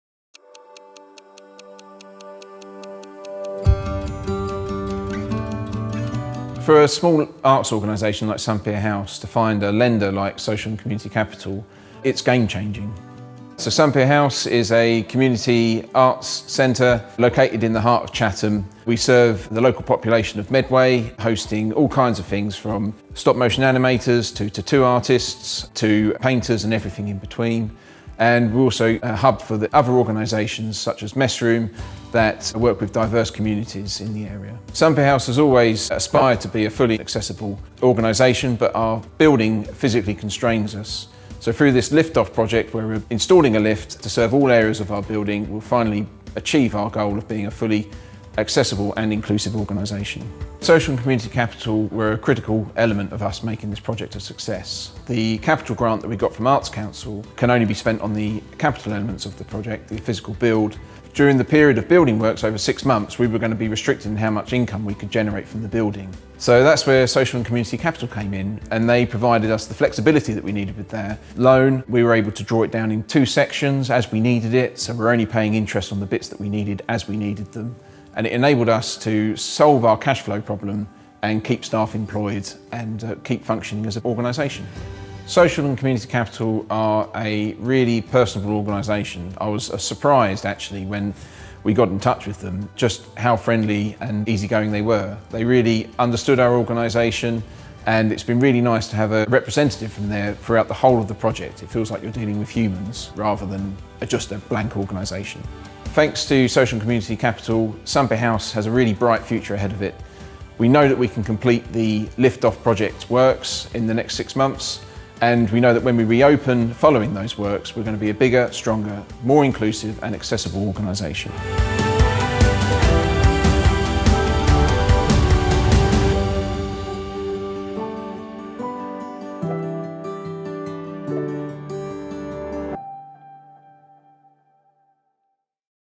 Sun Pier House CIC | UlsterBank Social & Community Capital
Sun Pier House CIC is a creative arts space and venue split across 3 floors with a big ambition: to enrich lives through arts and heritage.

They focus on supporting vulnerable people in Medway, one of the most deprived areas in the UK. Founded and run by Medway residents, Heather and Ed, Sun Pier House hosts an exhibition gallery, 26 artist studios, a community room, and a café. They work with artists, creative organisations, and anyone who wants to make the world better through cultural engagement.

Heather and Ed are committed to making a positive impact in Medway. Their vision is to make Sun Pier House a fully accessible and inclusive venue that everyone can enjoy.

To make Sun Pier House fully accessible, Ed and Heather launched ‘L!FT OFF,’ an ambitious funding project aimed at installing a lift in their building. The project secured a grant from the Arts Council. but there were still significant challenges. The building work was scheduled to take over six months, meaning reduced income in that time. They still had to pay bills and staff, creating a financial shortfall.

In search of a solution, Ed discovered Social and Community Capital (S&CC), who offered growth funding that was perfectly suited to their needs. “We opted for flexible growth funding to help us cover the shortfall’, Ed explains. “This allowed us to draw the loan in two sections and pay interest only on the amounts we needed and as we needed them.”

Ed was particularly pleased with S&CC’s friendly, empathetic approach: “They’re such a personable team. They really understood our organisation and it’s been so useful to have a representative from S&CC throughout our project.”

The funding from S&CC has been game changing. It solves Sun Pier House's cash flow problem and ensures that they could keep all their staff employed throughout the construction period. This financial stability allows Sun Pier House to continue hosting arts events and workshops. It’s meant they can provide training and support to local artists and build community participation through inclusive programmes.

Looking ahead, Sun Pier House plans to expand its programs and reach even more people. “Thanks to S&CC, Sun Pier House has a bright future ahead!” – Ed and Heather.

Read more about this story and how S&CC enables impact through its investees in our annual impact report